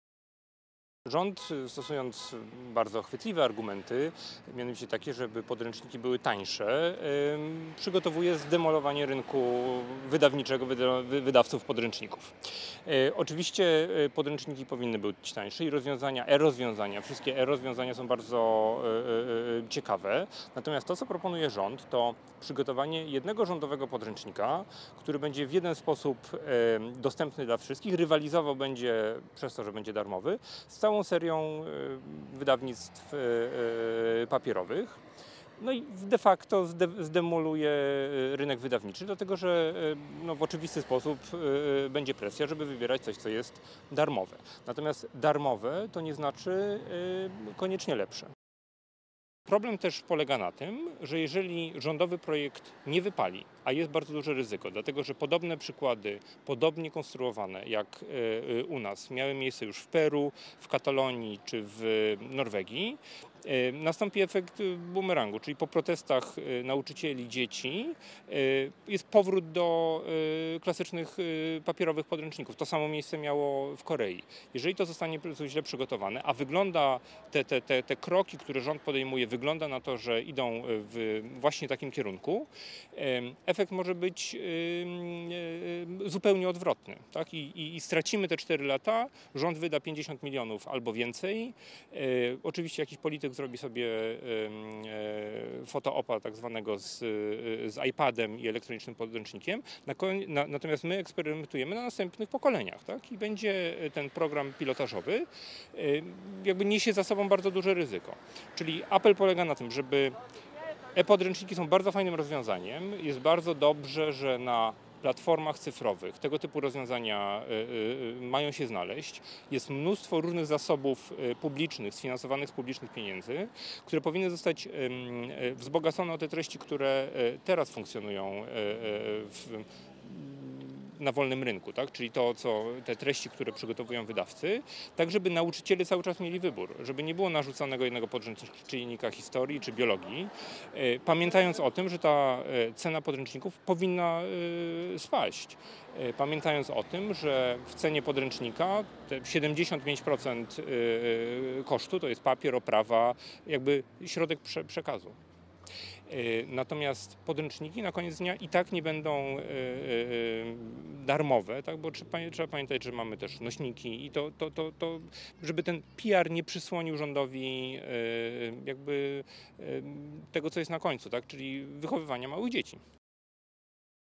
E podręczniki 2
Materiał Instytutu Jagielońskiego. Eksperci obawiają się, że rządowa propozycja wprowadzenia elektronicznego podręcznika nie sprawdzi się w praktyce.